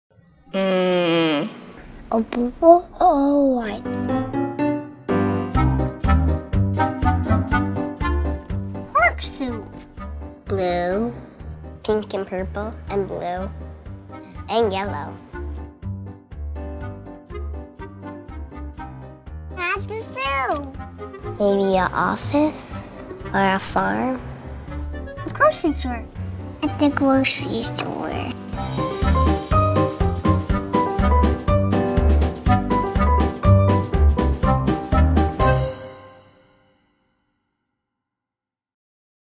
Where does a pharmacist go to work?
Ask a kid what they want to be when they grow up and you'll probably hear firefighter, astronaut, or teacher. But a pharmacist in specialty? What do they do? We asked several kids for their thoughts on the matter.